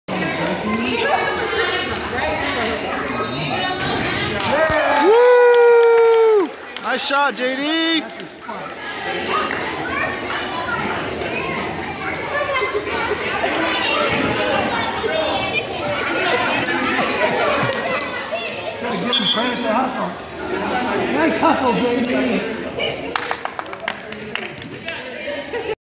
JD scores in his first basketball game. Feb 14, 2015
2-14-15  JD (Age 5) scores a basket in his first basketball game at Reeves Middle School.